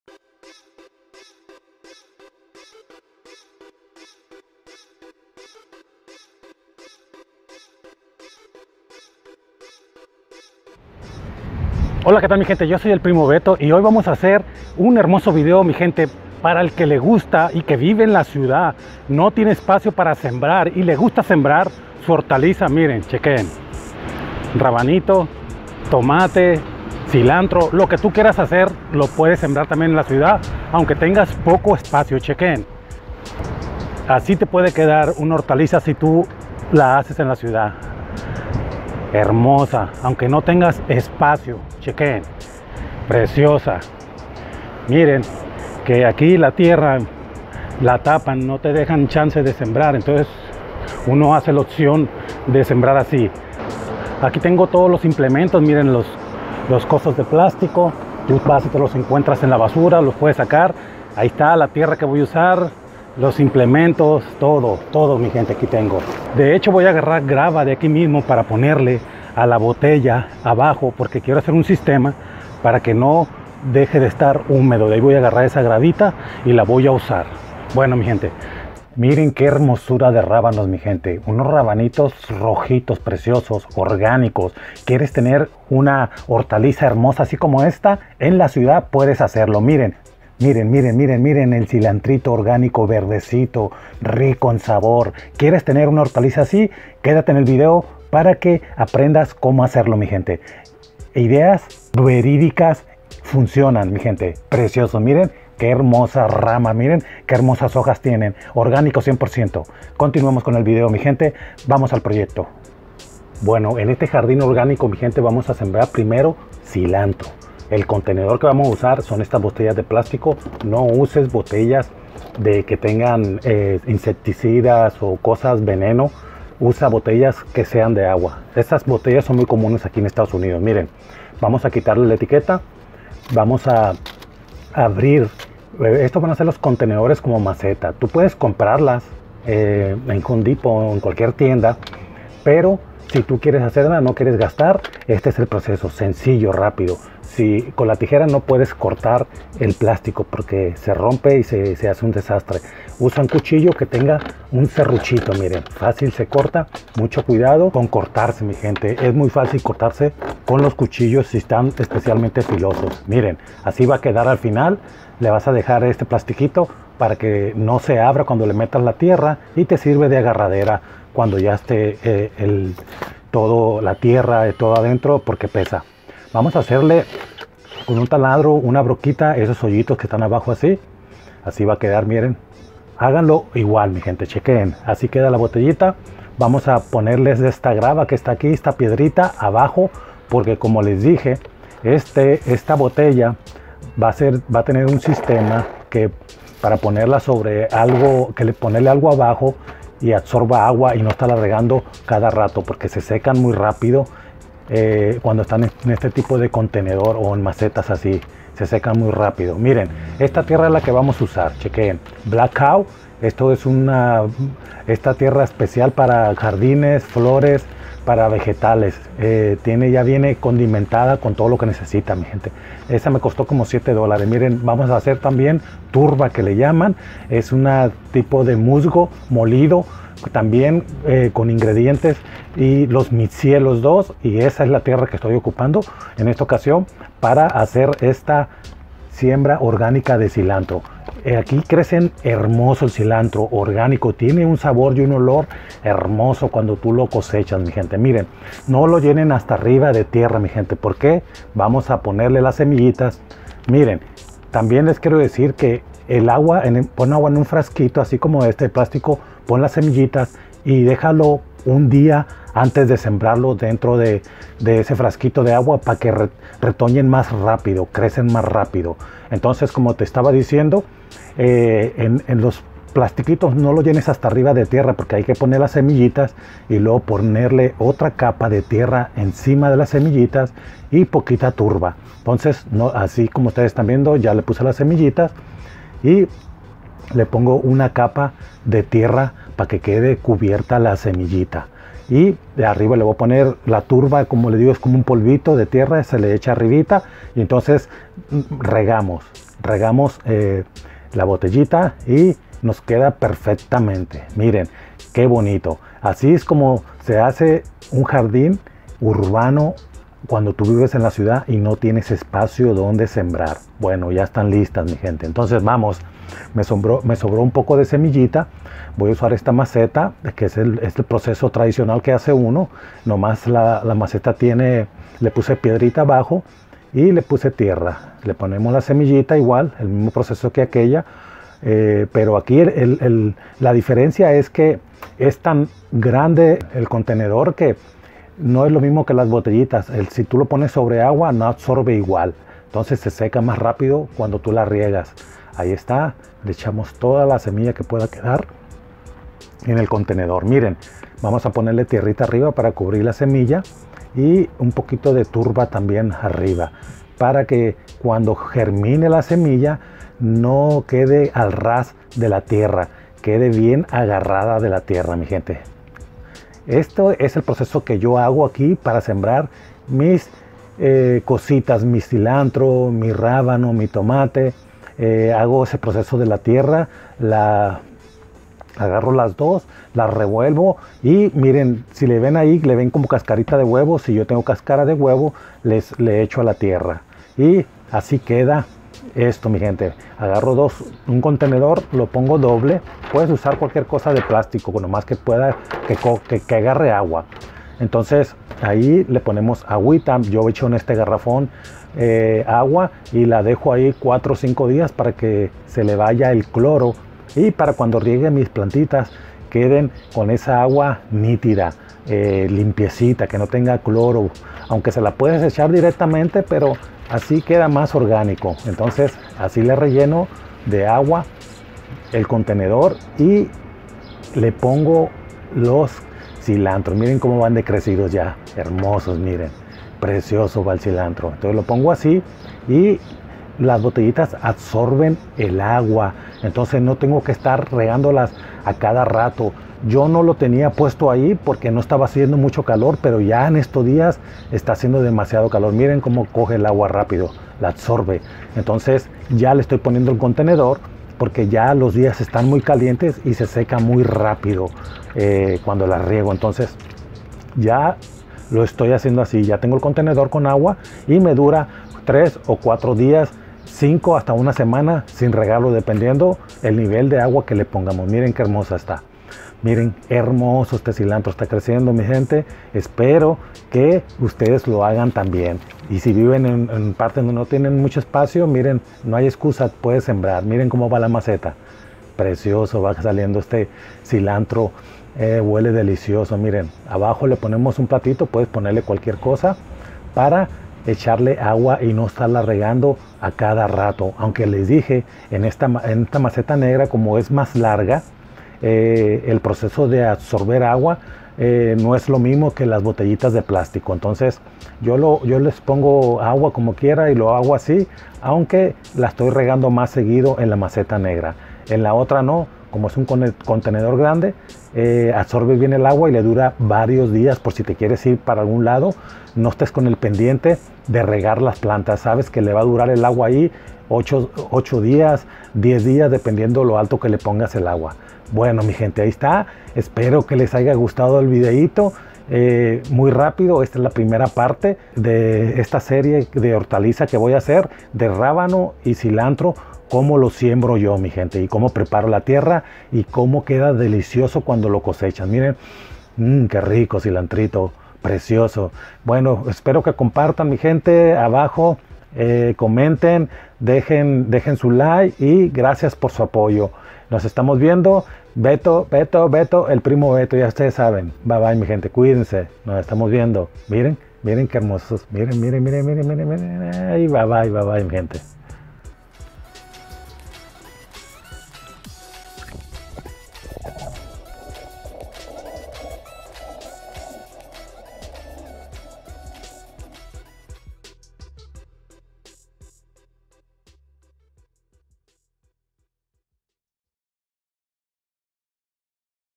⚠️ATENCION⚠️COMO SEMBRAR CILANTRO EN MACETA PARTE #1
HOLA QUE TAL MI GENTE EN ESTE VIDEO APRENDEREMOS A SEMBRAR CILANTRO EN CASA ORGANICO DELICIOSO EN TU HOHAR EN LA CIUDAD DONDE NO TENEMOS ESPACIO PARA SEMBRAR Y CON NUESTRO INGENIO SEMBRAMOS NUESTRA HORTALIZA COMO RABANO TOMATE CHILE DE TODO ESPERO LES GUSTE EL VIDEO COMPARTAN Y DALE LIKE